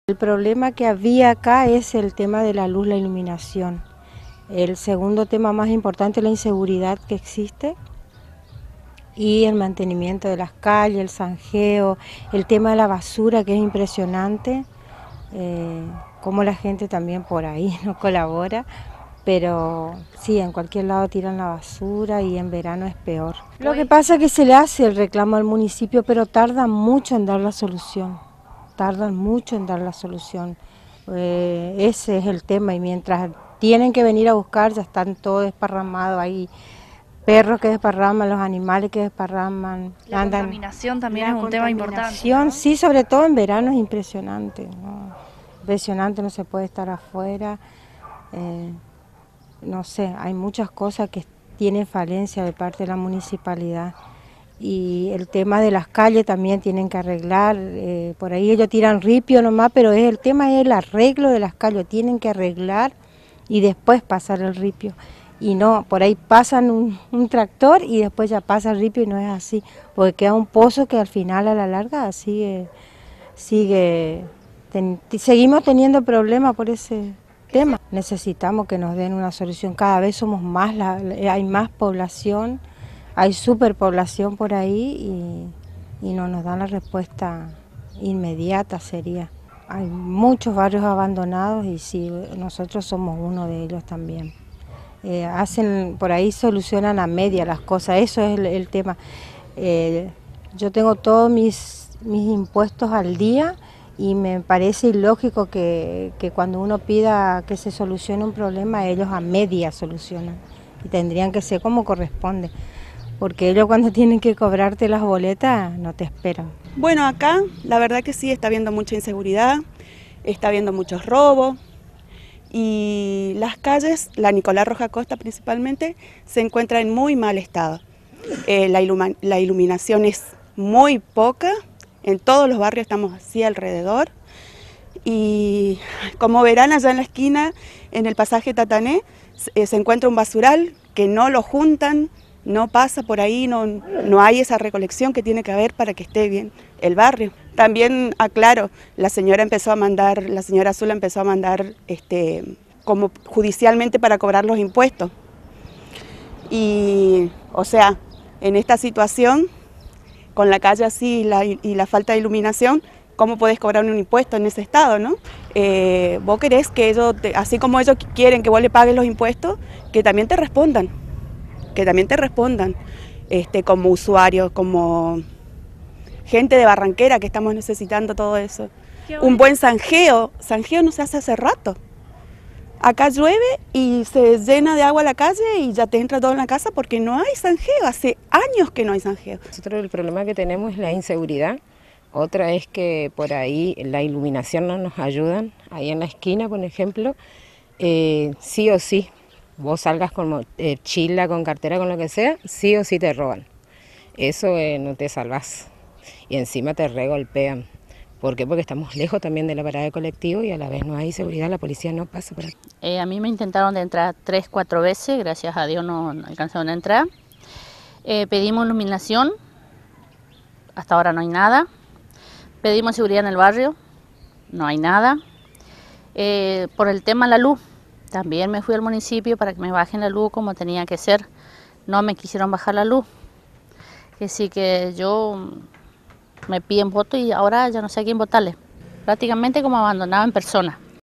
C9 - Chaco; Barranqueras abandonada
AUMENTAN LOS RECLAMOS DE LOS VECINOS
Vecinos del Barrio 237 viviendas de la ciudad de Barranqueras, apuntaron contra el municipio por el estado de abandono del lugar. “Hace años venimos pidiendo iluminación, seguridad y limpieza y no nos dan soluciones” indicó una vecina